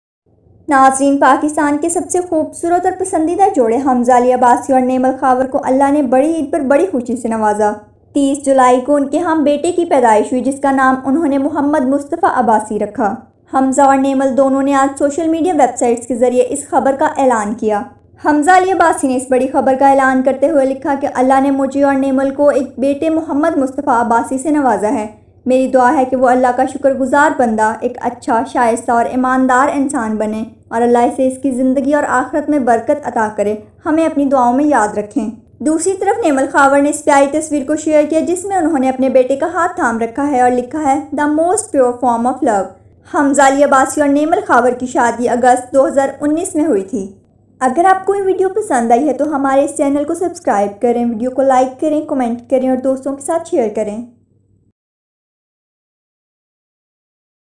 Pakistan's Most Beautiful Couple Hamza Abbasi and Naimal Khawar Gave Birth To Baby Boy | News Today
Pakistan's Most Beautiful Couple Hamza Ali Abbasi and Naimal Khawar Gave Birth To a Baby Boy | News Today